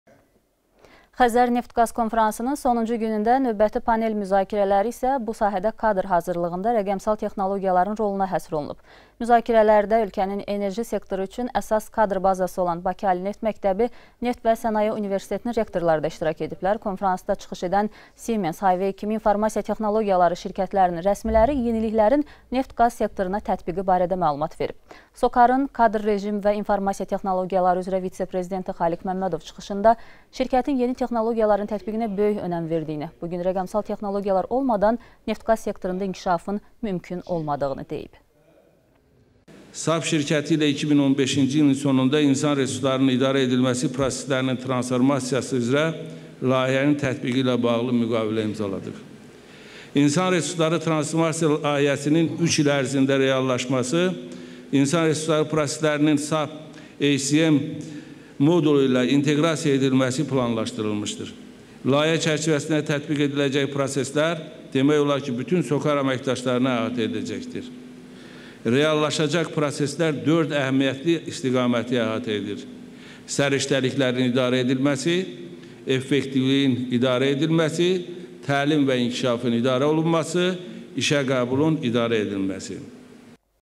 NEFT-QAZ SEKTORUNDA KADR HAZIRLIĞINDA SOCAR YENİ TEXNOLOGİYALARIN TƏTBİQİNƏ ÖNƏM VERİR
Xəzər Neft-Qaz konfransının sonuncu günündə növbəti panel müzakirələri isə bu sahədə kadr hazırlığında rəqəmsal texnologiyaların roluna həsr olunub. Müzakirələrdə ölkənin enerji sektoru üçün əsas kadr bazası olan Bakı neft məktəbi, Neft və Sənaye universitetinin rektorları da iştirak edib. Konfransa çıxış edən Simens, Huayvey kimi İT şirkətlərin rəsmiləri yeniliklərin neft-qaz sektoruna tətbiqi barədə məlumat verib. SOCAR-ın kadr,rejim və informasiya texnologiyaları  üzrə vitse-prezidenti Xalik Məmmədov çıxışında şirkətin yeni texnologiyaların tətbiqinə böyük önəm verdiyini, bu gün rəqəmsal texnologiyalar olmadan neft-qaz sektorunda inkişafın mümkün olmadığını deyib.